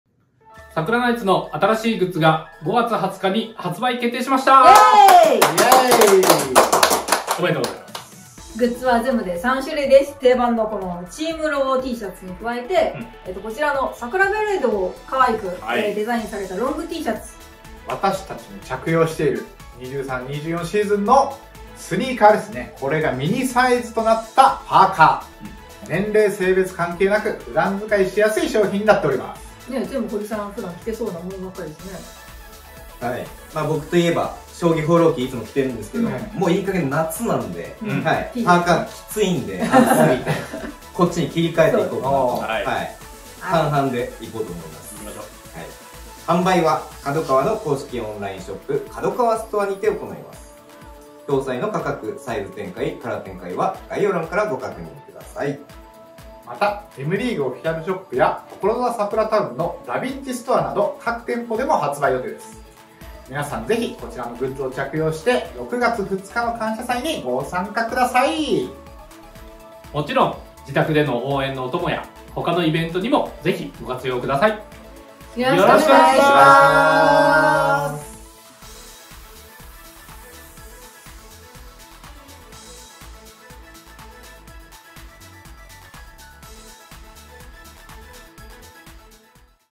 サクラナイツ2023-24シーズン夏グッズ告知動画
【5/20(月) 0時】からサクラナイツの新グッズが発売決定！🌸

定番の「チームロゴTシャツ」や4色の「サクラブレードのロンT」、
今シーズン選手着用の「スニーカーパーカー」など
どなたでも着こなしやすいグッズです！✨

KADOKAWA公式オンラインショップの「カドカワストア」から
5/20(月) 0時以降に下記URLから各商品ページで購入可能！🌸

また、「Mリーグオフィシャルショップ」、
ところざわサクラタウン内「ダヴィンチストア」の店頭でも発売予定！✨
ぜひこちらを着用して6月2日(日)の感謝祭や
各種イベントへの参加および桜援のお供にご活用ください！

詳細はこちらのページから！